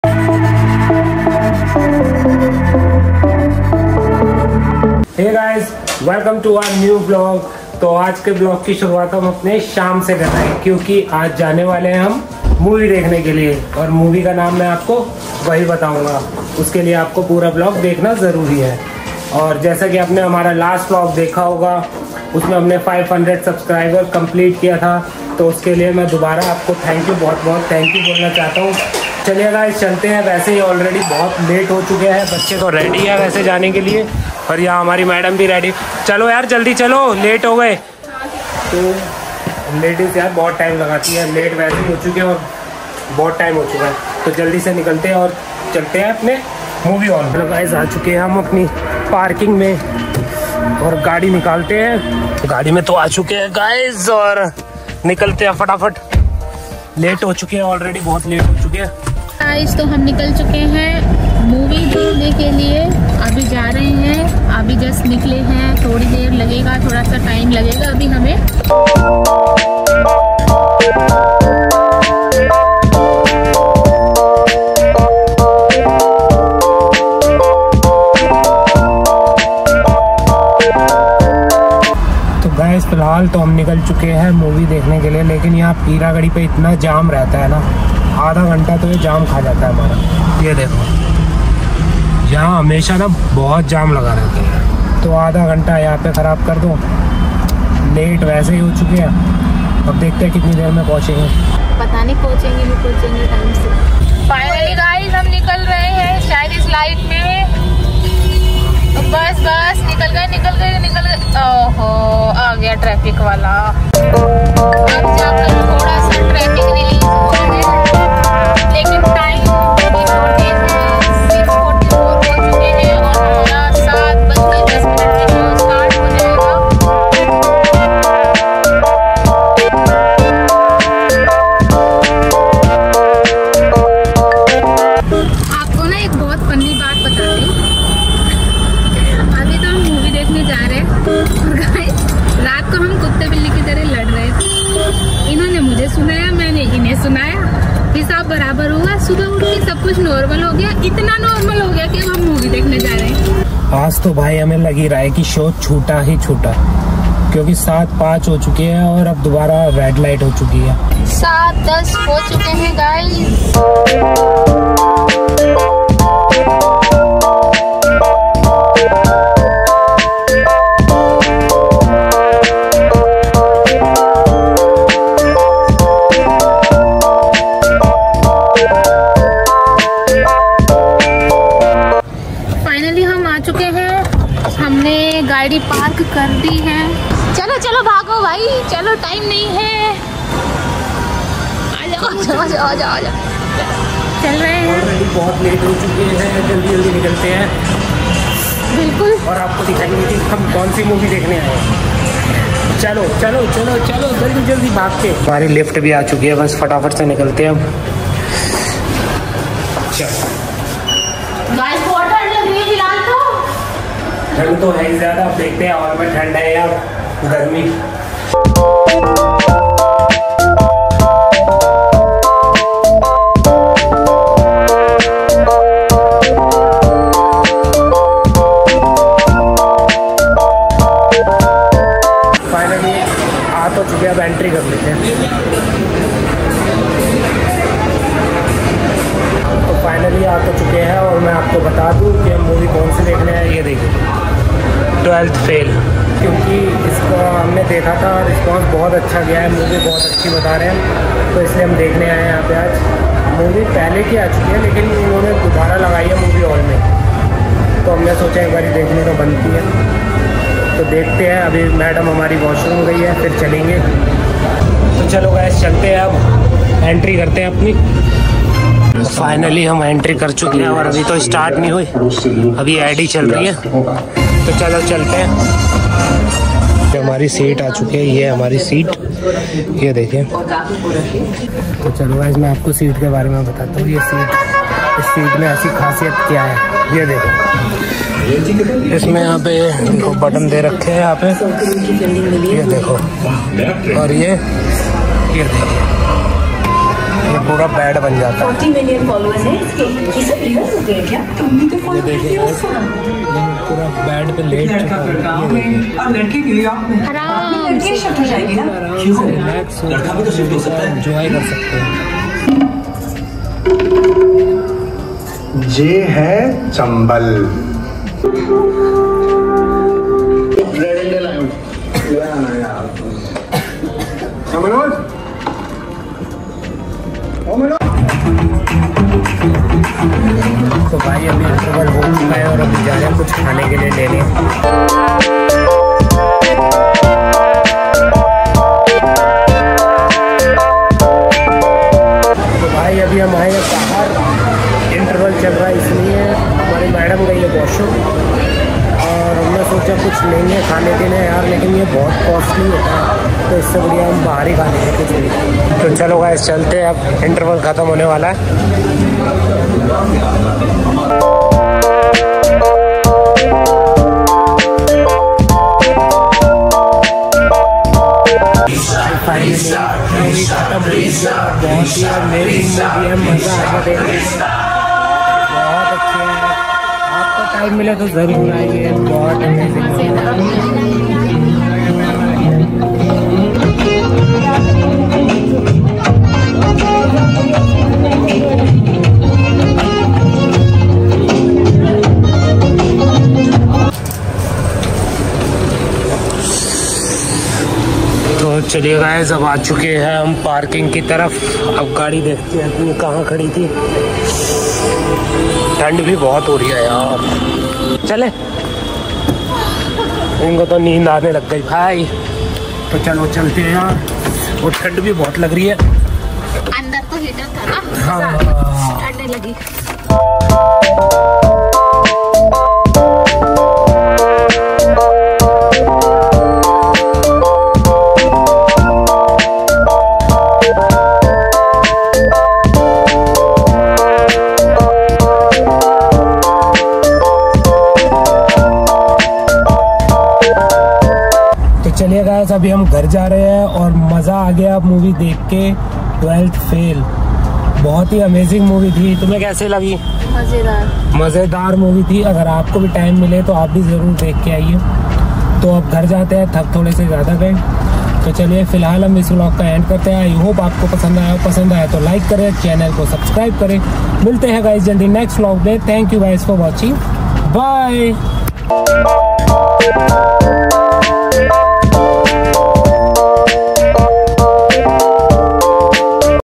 12th Fail Movie Dekhne Gaye Dobara...(Vlog 19) || Dilip || Gayatri || Tushar || DGT Vlogs
Hello Everyone.

I hope ap sabhi bhot ache honge. Aj is vlog me hum gaye 12th movie dekhne dobara . Is vlog me apko dikhega ki hum bhot late ho chuke the movie ke show ke liye. Video ko last tak zaroor dekhna.